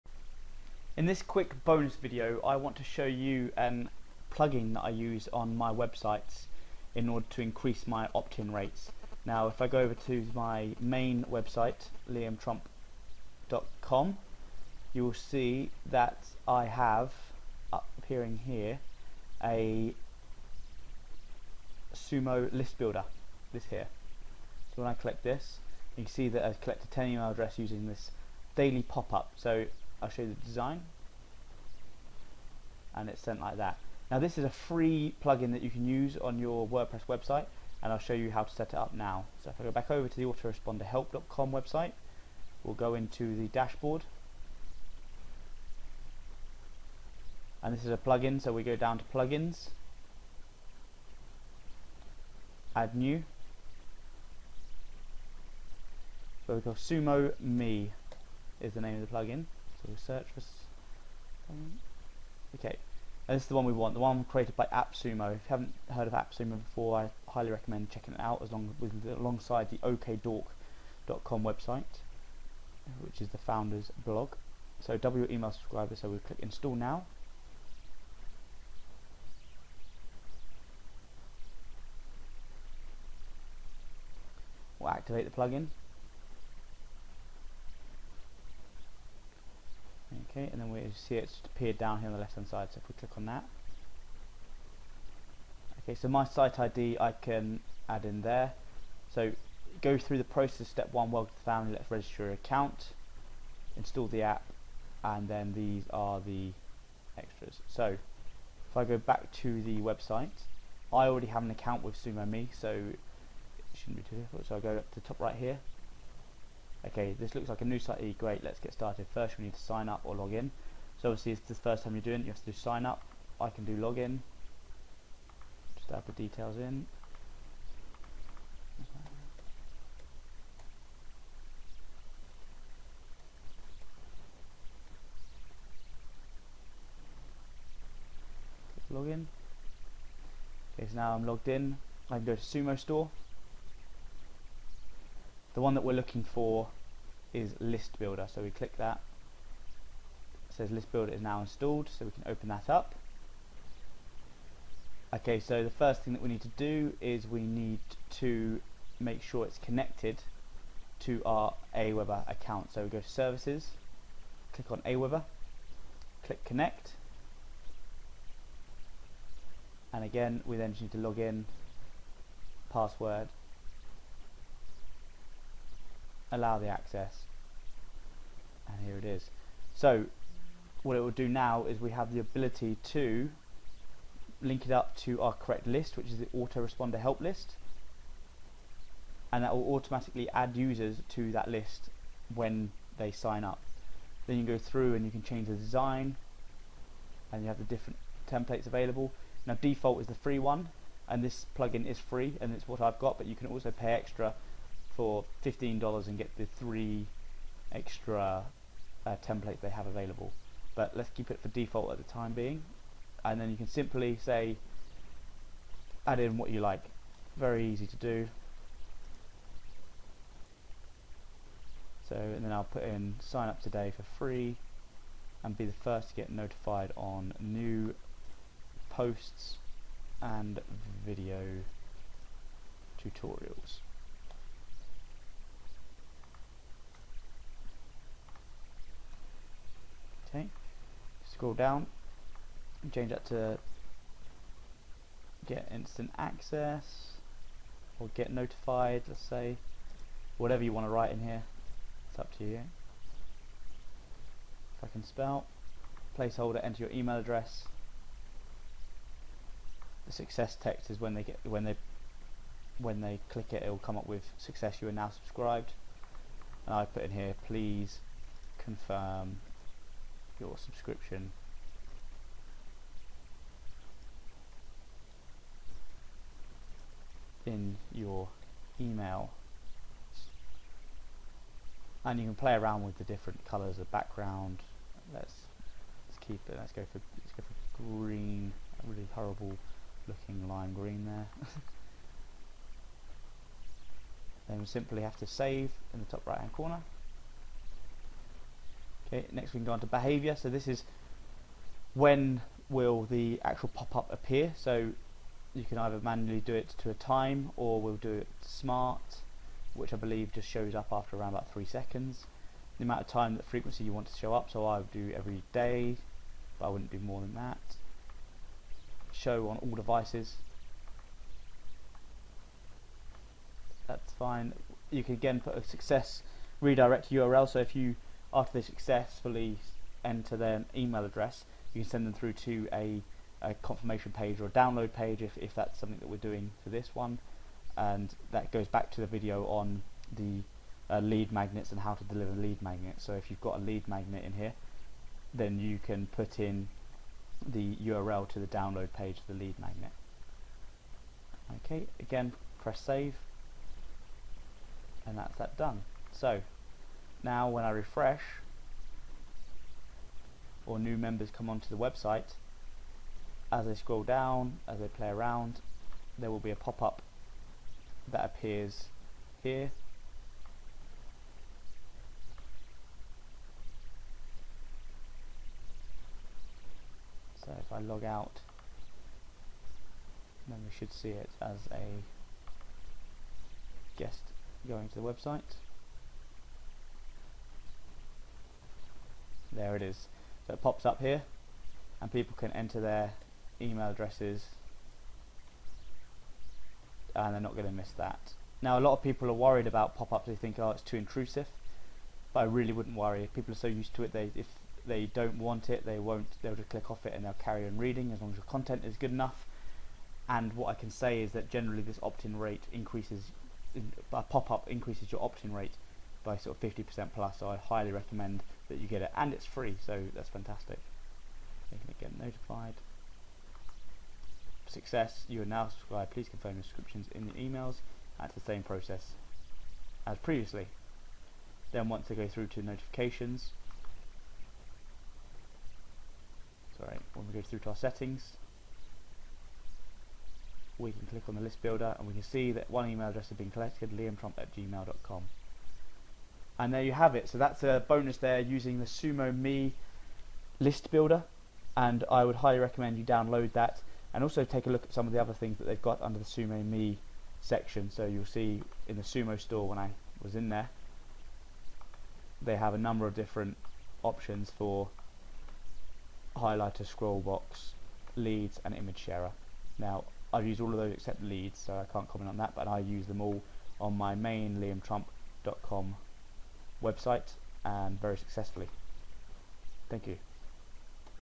Adding The Free SumoMe Plugin To Increase Website Opt-Ins
Building Your Email List Is Critical In Online Business. Watch this video to see how you can add the free SumoMe plugin to your Wordpress website to increase opt-ins.